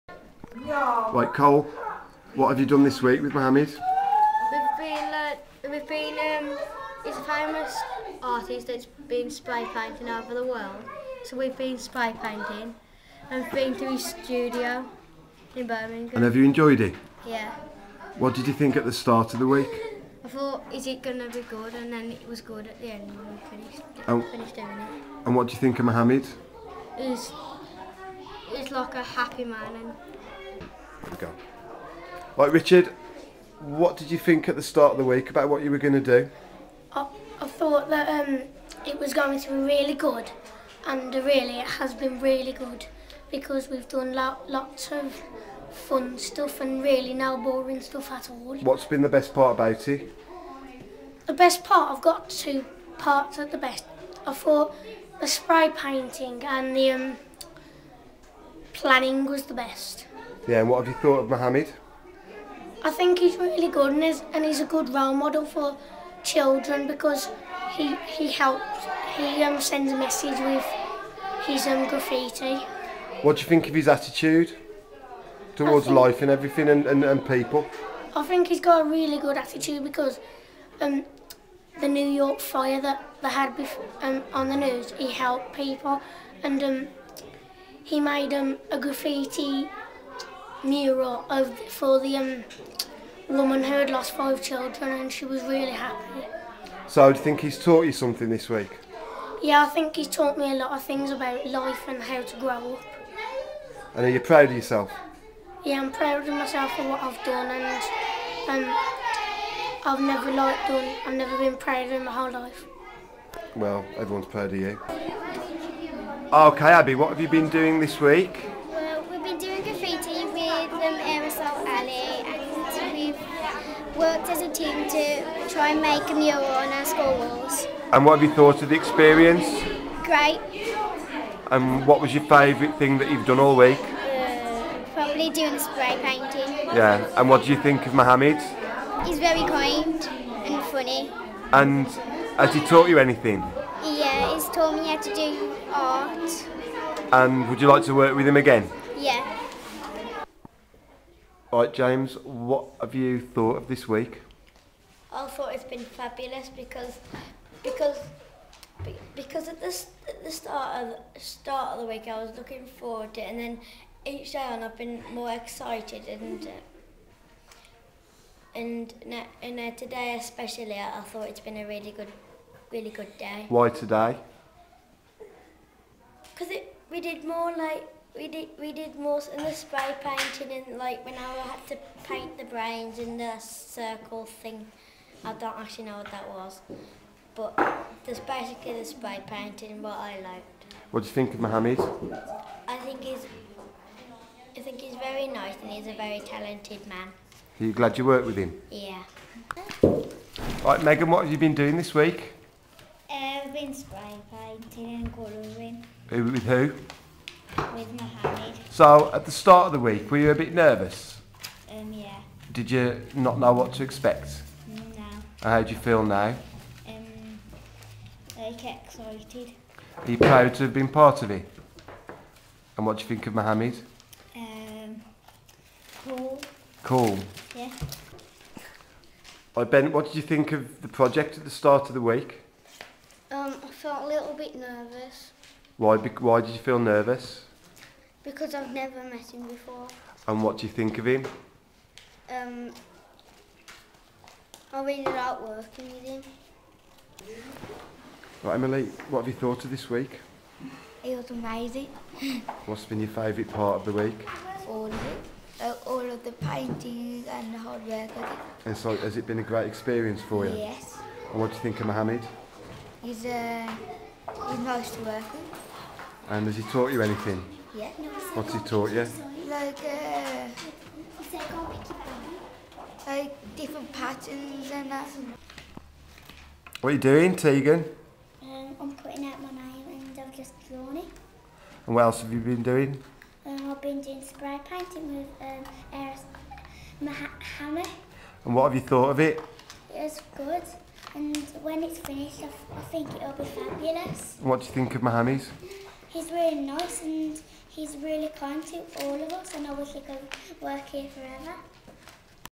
Mural project at Whitgreaves Primary School
some voxpops with young people at Whitgreaves Primary